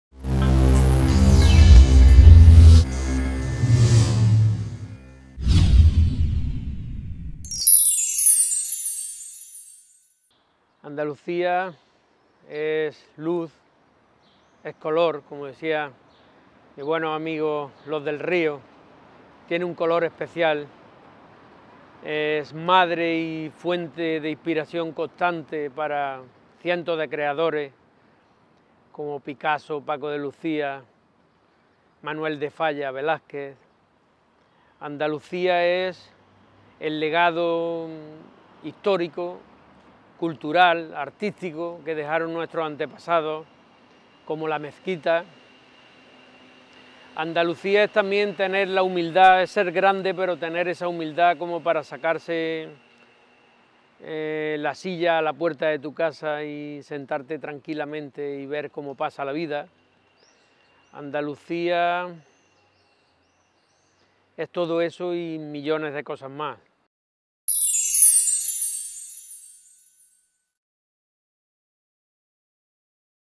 HDL El granito de luz del productor musical Manuel Ruiz 'Queco'
Para el productor musical y cantante cordobés Manuel Ruiz 'Queco' Andalucía es "luz y color",  es  "madre y fuente de inspiración constante" para cientos de creadores como Picasso, Paco de Lucía, Manuel de Falla, Velázquez: "es el legado histórico, cultural y artístico que dejaron nuestros antepasados".

asegura que Andalucía es y ha sido fuente de inspiración para grandes creadores como Paco de Lucía, Manuel de Falla o Velázquez. Es el legado histórico que nos dejaron nuetsros antepasado y también la humildad de sus gentes. Todo eso y un millón de cosas más.